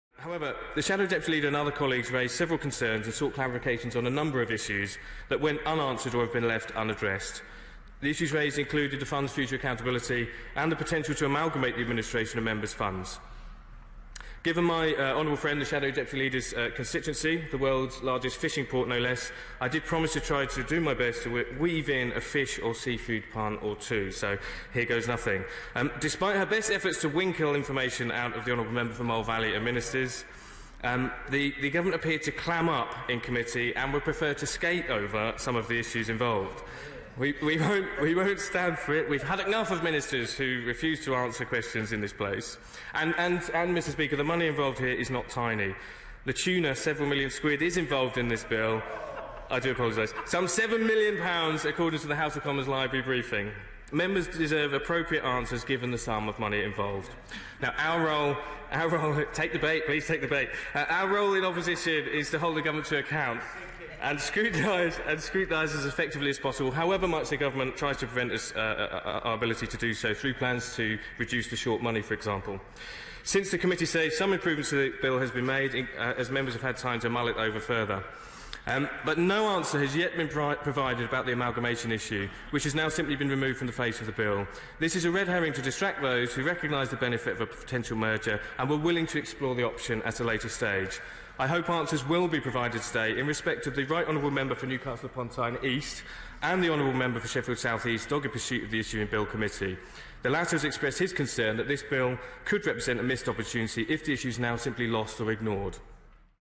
MP Neil Coyle sneaks fish puns into House of Commons speech - Daily Mail
Labour MP for Bermondsey and Old Southwark, Neil Coyle, managed to weave in at least ten fishy puns in a House of Commons speech today. He was standing in for Melanie Onn, MP for Grimsby and Shadow Deputy Leader of the House of Commons. He told his fellow MPs that as her constituency had the world's largest fishing port, he had promised to include as many fish and seafood puns in his speech as possible.